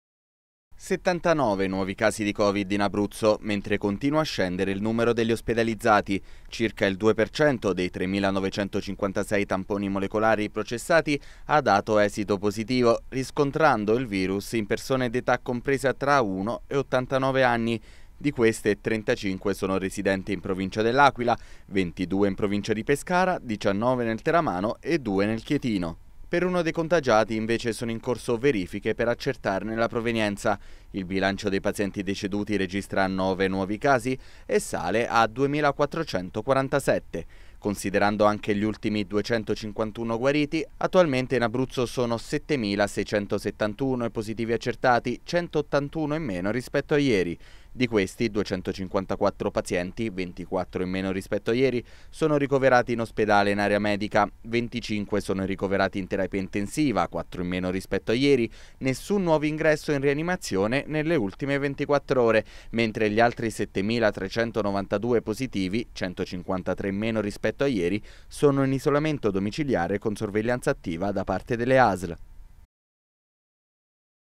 Covid Abruzzo. 79 casi e nove morti, diminuiscono gli ospedalizzati
Bollettino Covid: oggi in Abruzzo 79 nuovi casi (1,9%), nove morti e 251 guariti. Continua a scendere il numero complessivo dei ricoverati.